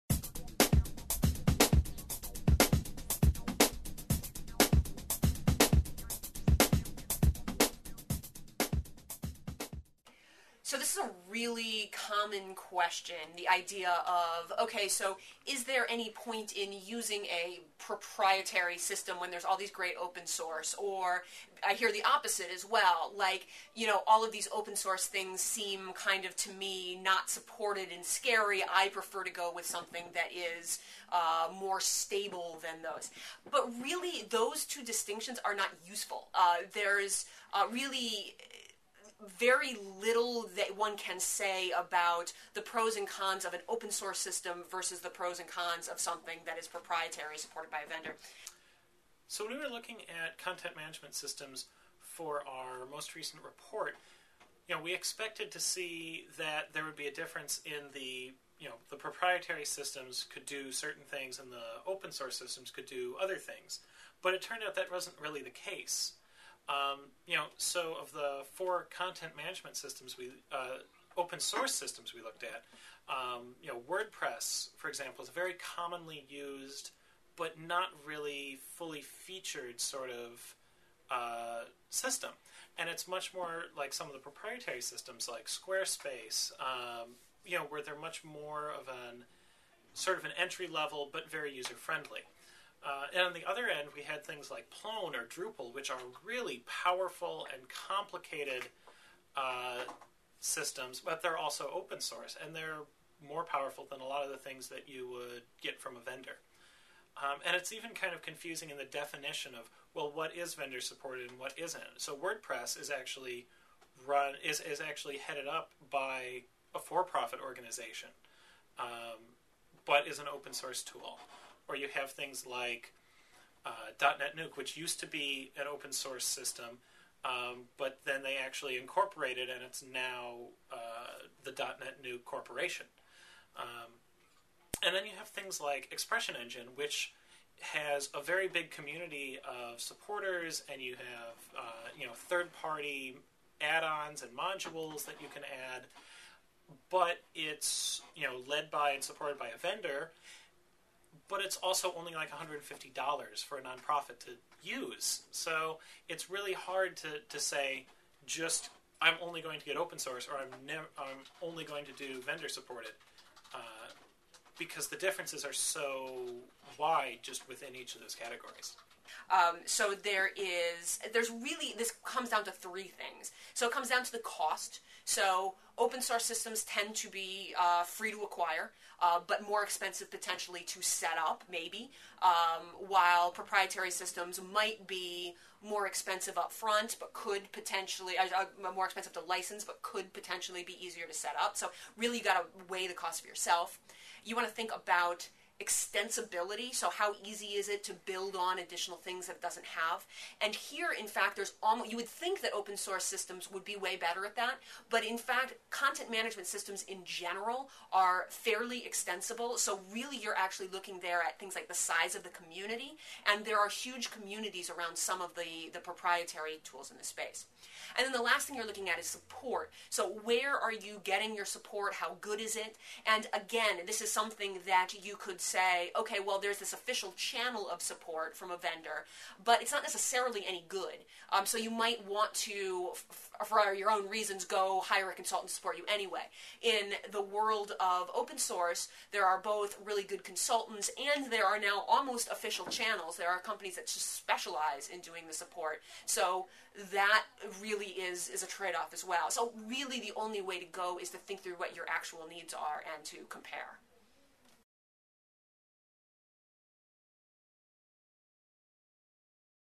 Should I use an open-source system to manage my website?
What's better, an open-source CMS, or one supported by a vendor? Is there a difference?

Laura Quinn and Kyle Andrei explain how that's not really the question you should be asking.